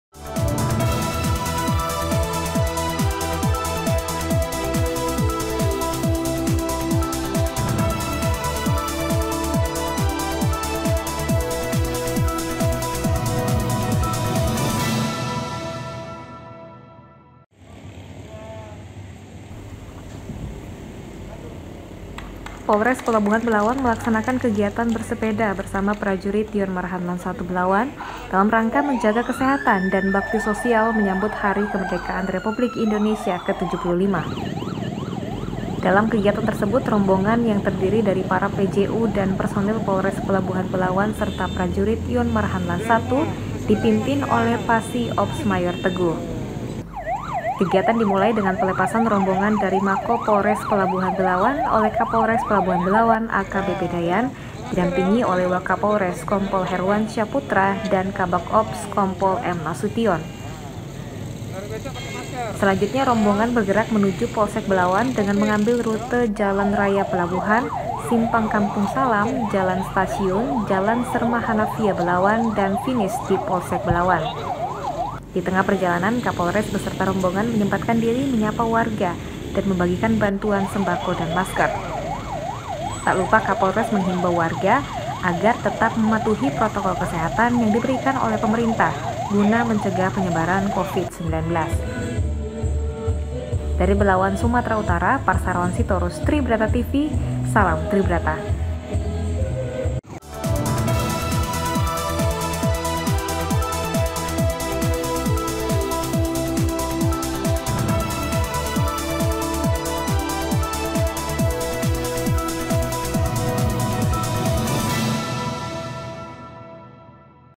GOWES POLRES BELAWAN BERSAMA YONMARHANLAN 1 SEKALIGUS BAGI BAGI SEMBAKO DAN MASKER
Polres Pelabuhan Belawan melaksanakan kegiatan bersepeda bersama prajurit Yon Marhanlan 1 Belawan dalam rangka menjaga kesehatan dan bakti sosial menyambut hari Kemerdekaan Ke 75 Republik Indonesia.

Dalam kegiatan tersebut rombongan yang terdiri dari para PJU dan Personil Polres Pelabuhan Belawan serta prajurit Yon Marhanlan 1 Belawan yang dipimpin oleh PASI OPS Mayor Teguh.

Kegiatan dimulai dengan pelepasan rombongan dari Mako Polres Pelabuhan Belawan oleh Kapolres Pelabuhan Belawan AKBP Dayan yang di dampingi Wakapolres Kompol Herwansyah Putra dan Kabag Ops Kompol M.Nasution.

Selanjutnya rombongan bergerak menuju Polsek Belawan dengan mengambil rute Jalan Raya Pelabuhan, Simpang Kampung Salam, Jalan Stasiun, Jalan Serma Hanafiah Belawan dan finish di Polsek Belawan.

Di Tengah Perjalanan Kapolres beserta rombongan menyempatkan diri menyapa warga dan membagikan bantuan sembako dan masker kepada para abang becak yang sedang menunggu penumpang di pinggir jalan.

Tak lupa Kapolres menghimbau warga dan para abang becak untuk tetap mematuhi protokol kesehatan yang diberikan oleh pemerintah guna mencegah penyebaran covid-19.

Dari Belawan Sumatera Utara Parsaoran Sitorus Tribrata Tv Salam Tribrata.